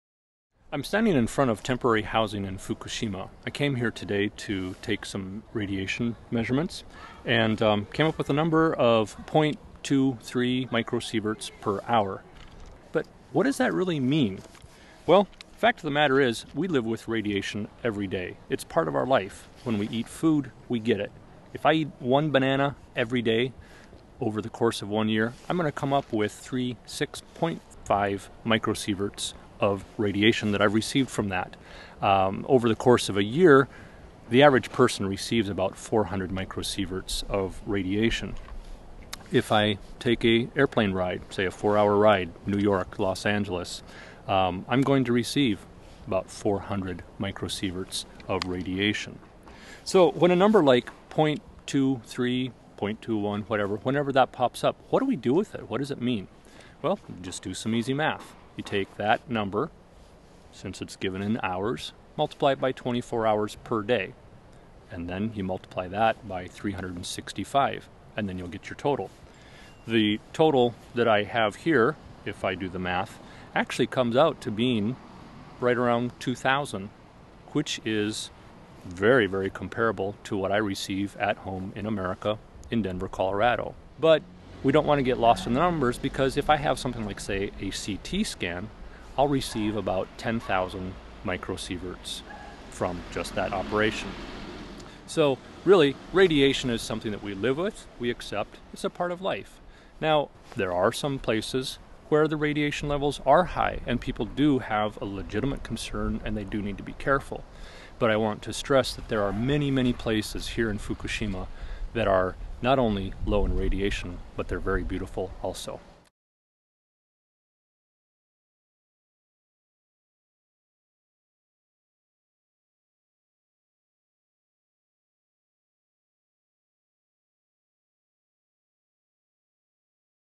Japan Radiation Safety 日本放射の安全
（日本語は英語の後にあります）As CRASH Japan moves towards its second year of serving survivors of the March 11 disaster, there are still plenty of opportunities for volunteers. CRASH is taking measures to ensure the safety of volunteers, including the recent addition of safety chief Nathan Williams to the headquarters staff. Among many places in the disaster zone, Nathan recently visited a temporary housing site in Fukushima City where he measured environmental radiation levels. CRASH Japan is committed to mobilizing volunteers safely and effectively. Please pray, give, and/or volunteer today. For more information on radiation safety in Japan we recommend the following

クラッシュジャパンは3月11日の震災被災者を援助する、2年目の年に入りました。まだまだ、ボランティアの活躍できる機会があります。クラッシュは安全管理主任ナタン・ウイリアムスを本部に迎え、ボランティアの安全を確保するため放射線の測定を始めました。環境における放射線量を測定するため、ナタンは最近、福島市の仮設住宅を訪問しました。クラッシュジャパンは安全に、かつ効果的にボランティアを派遣することに献身しています。是非、祈り、捧げ、またボランティアしてください。日本での放射線安全基準について、さらに詳しくお知りになりたい方は、こちらをご覧下さい。(英語のみ)